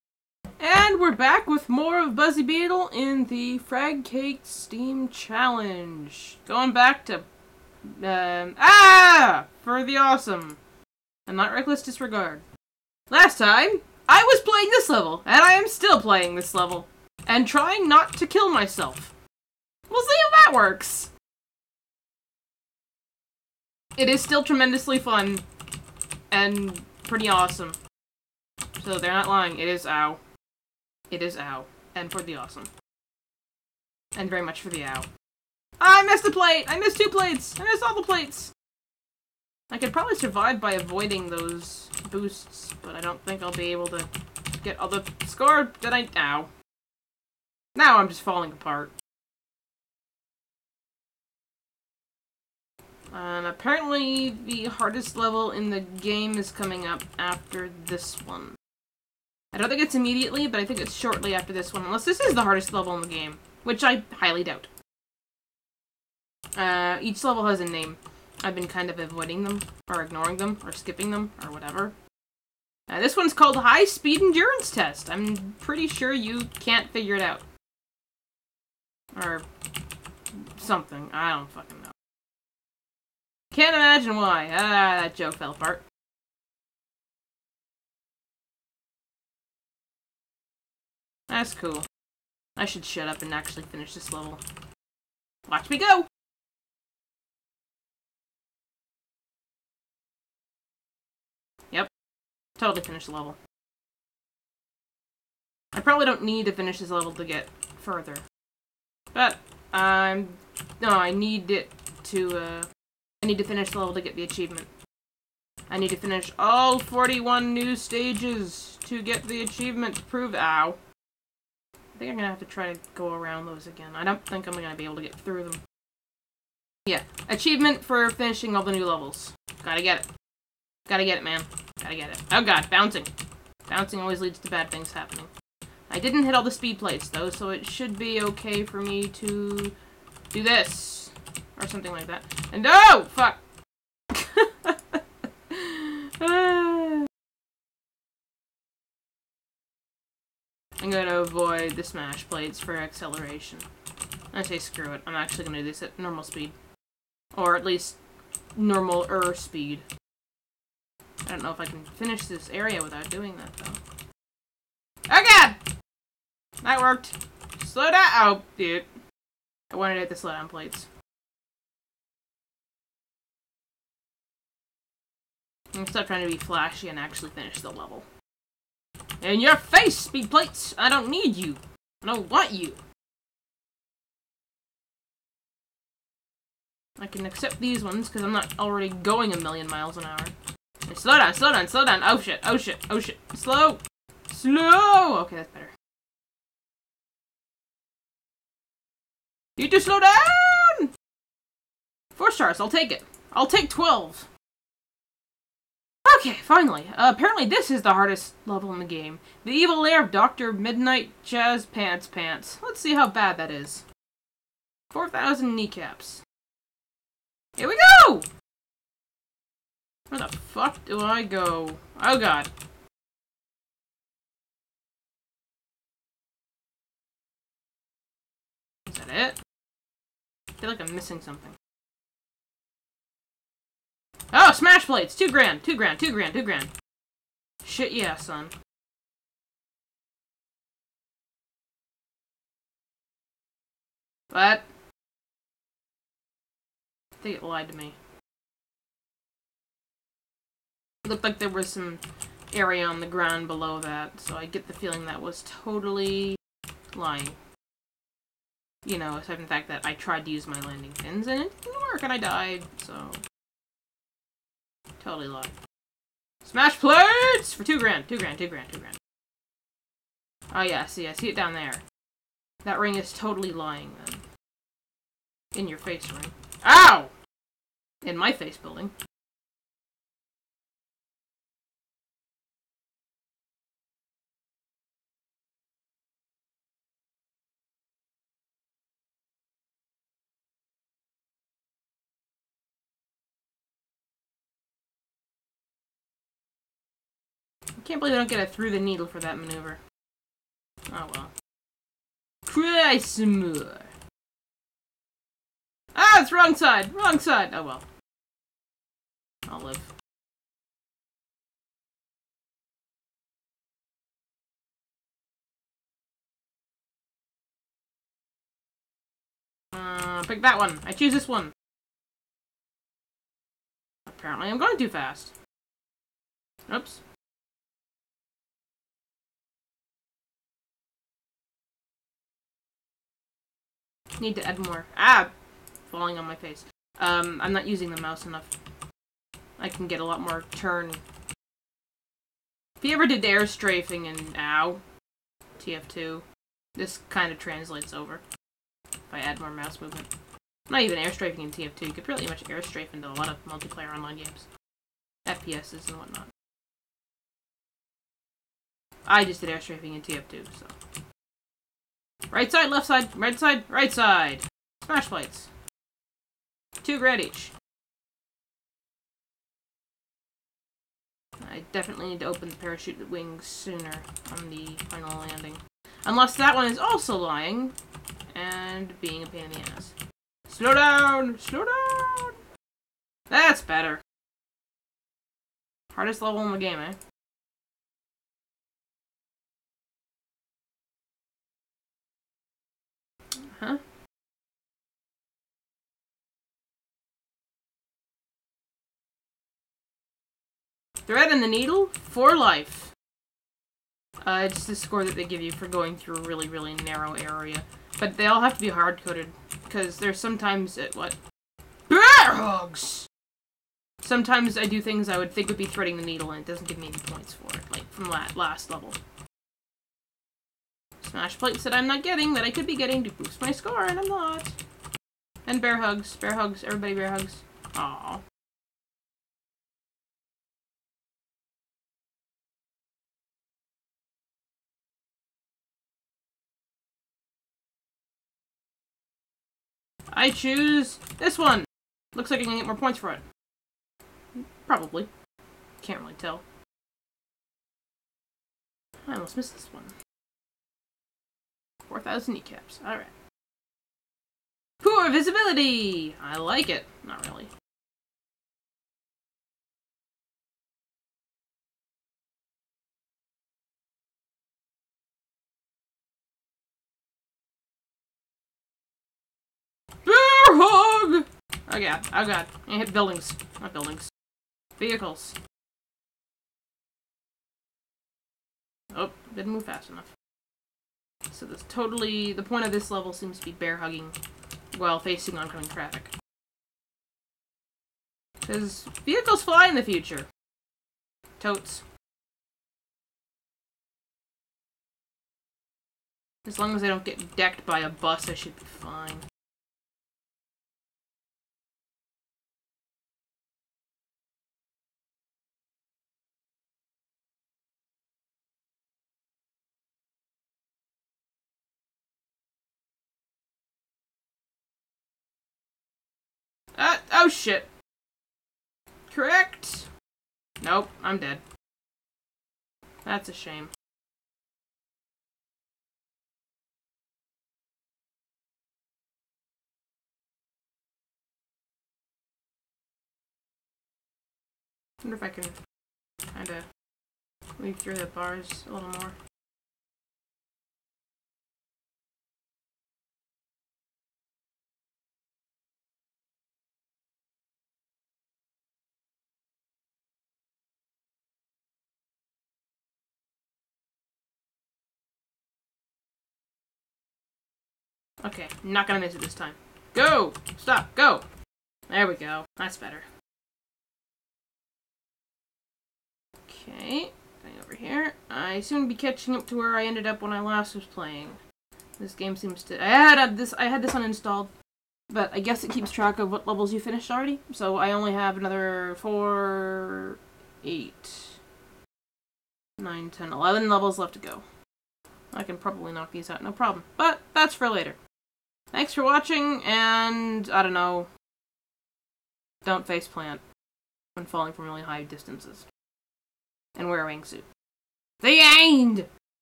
FragCake Challenge: AaaaaAAaaaAAAAAA!!! for the Awesome! Pt 3
More High-Speed Endurance Test and high-speed human sidewalk paste!

Sorry there's no game sounds, FRAPS decided to misbehave and not record them.